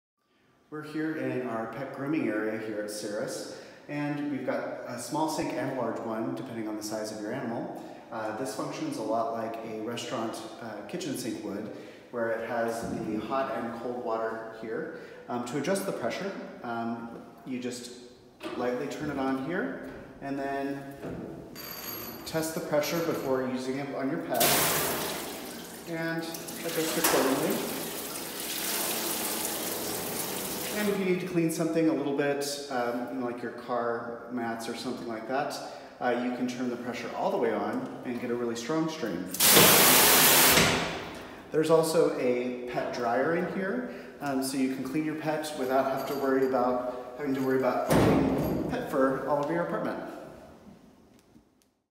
Pet Grooming Station Instructions
A tutorial for our Pet Grooming Station at Cirrus.